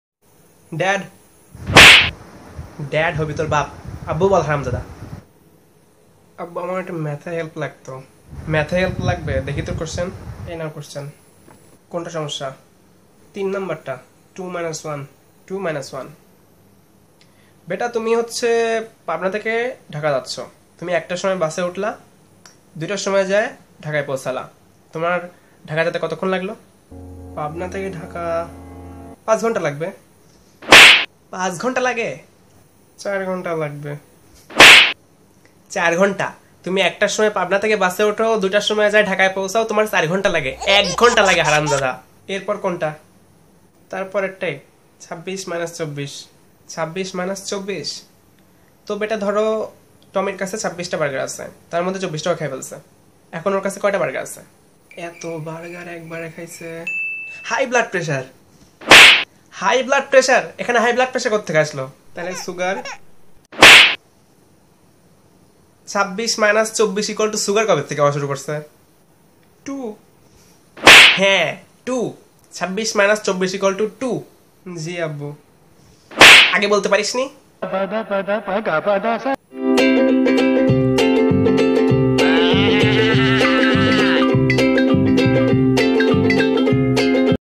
Tomato vines || Abbu explains mathematics problem
Presenting : How my father explains me math problems. Watch the video. Enjoy with fun .